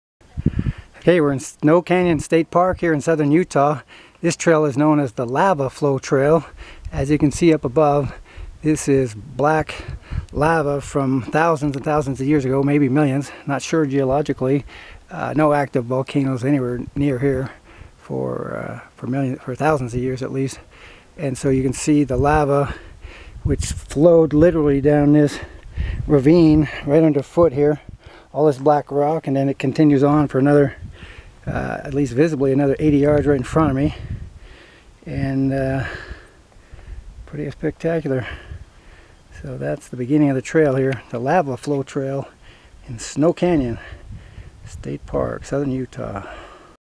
Snow Canyon State Park, Utah - The Lava Flow Trail
Interesting flow of lava rock down the ravine and along the trail.